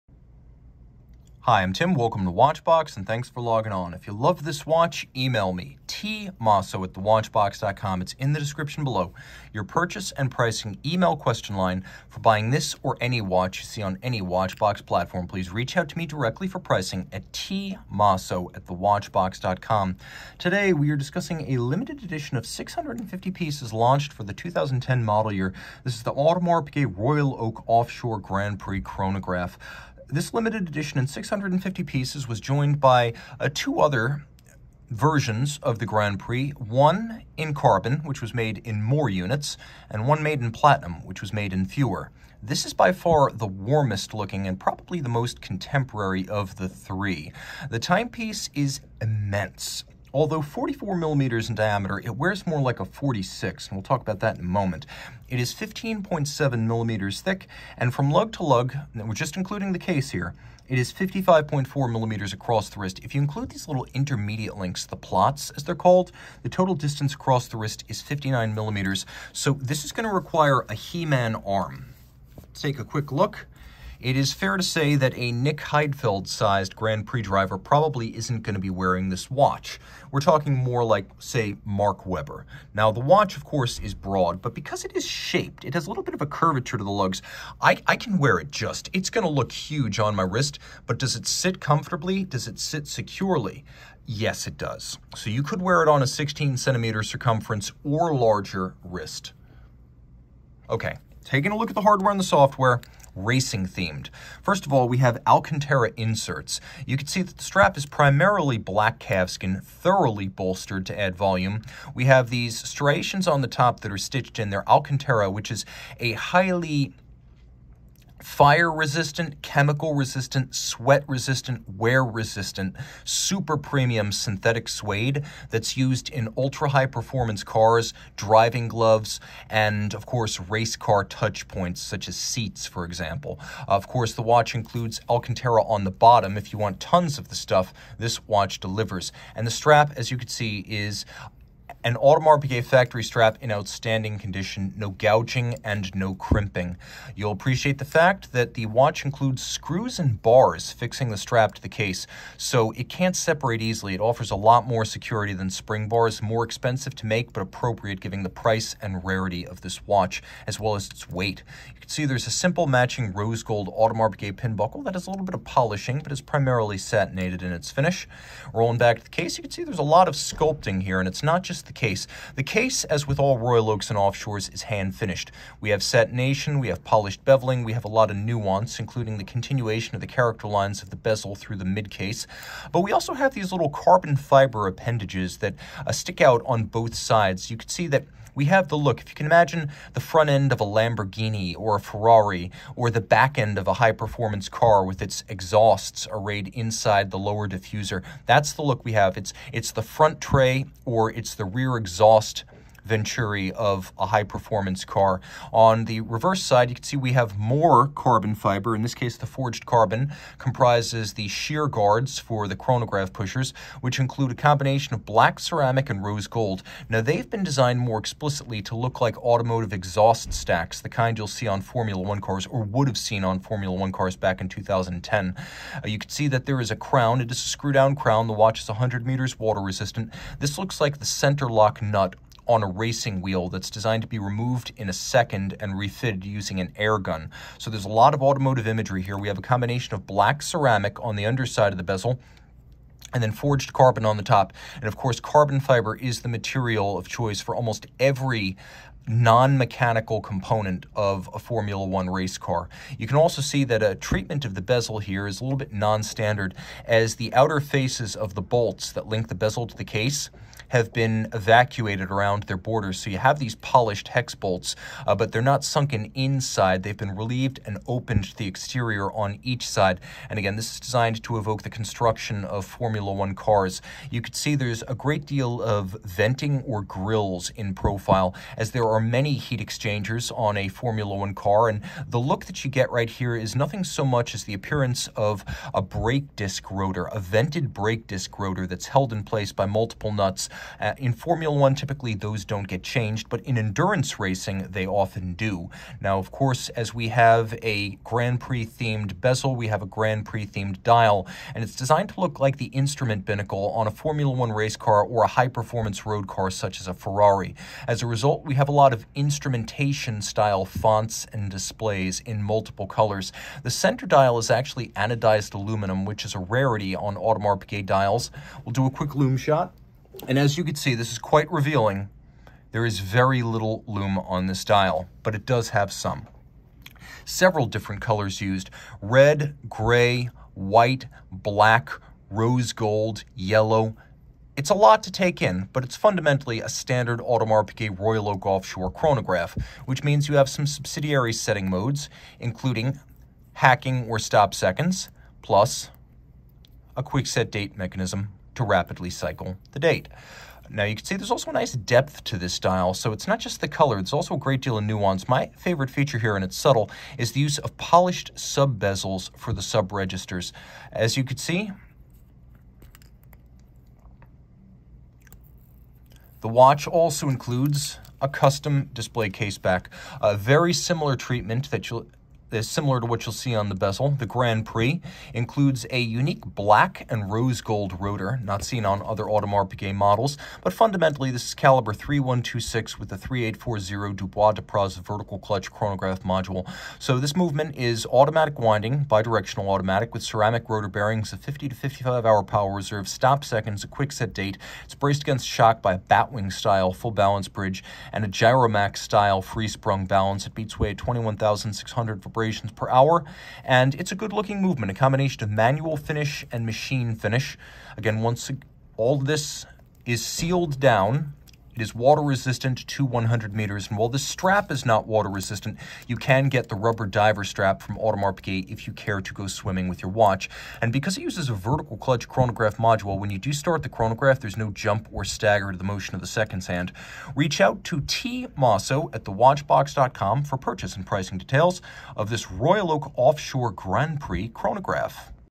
Audemars Piguet Royal Oak Offshore Grand Prix Chronograph 26290RO Audemars Piguet Review
SKU: 4607826

The Audemars Piguet Royal Oak Offshore Grand Prix Chronograph Limited Edition 26290RO.OO.A001VE.01 is encased in 44mm of 18k rose gold surrounding a black Mega Tapisserie dial on a black leather strap. Features of this Audemars Piguet Royal Oak Offshore Grand Prix Chronograph Limited Edition include hours, minutes, small seconds, date and chronograph.

For complete details, watch the full Audemars Piguet Royal Oak Offshore Grand Prix Chronograph Limited Edition review by Tim Mosso!